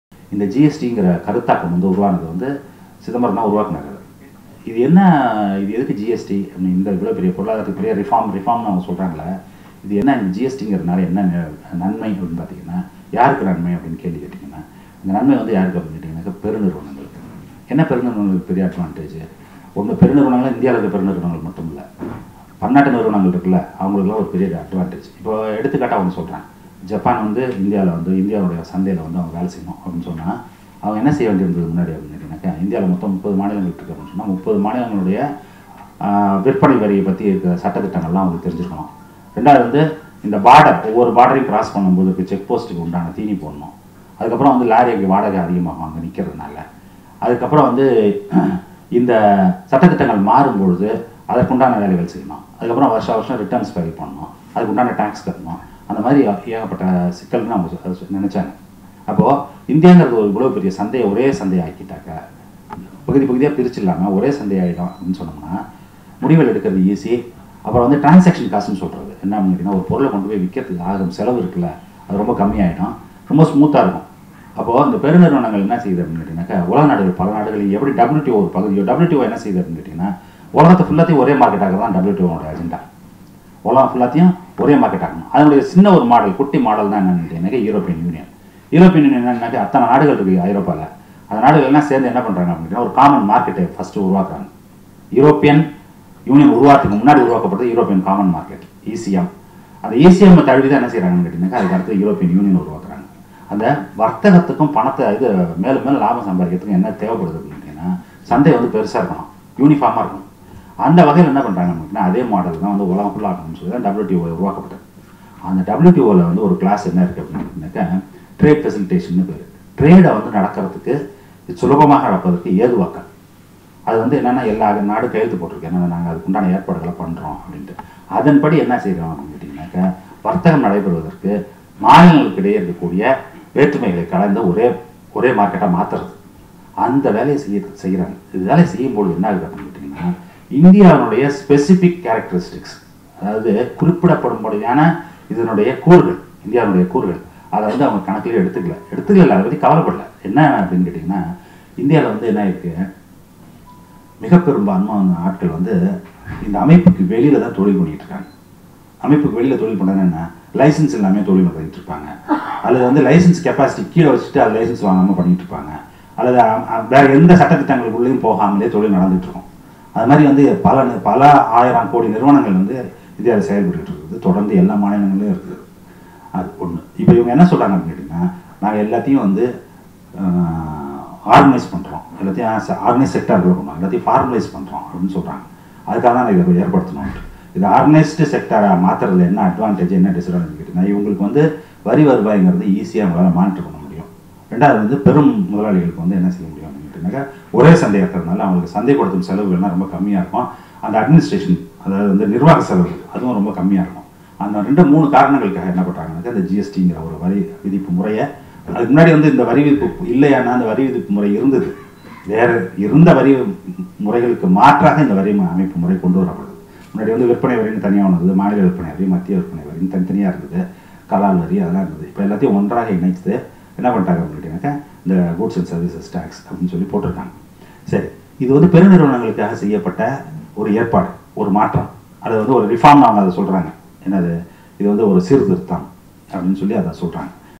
இந்தியா - GST யினால் யாருக்கு லாபம் | ஜெ.ஜெயரஞ்சன்
பொருளாதார வல்லுநர்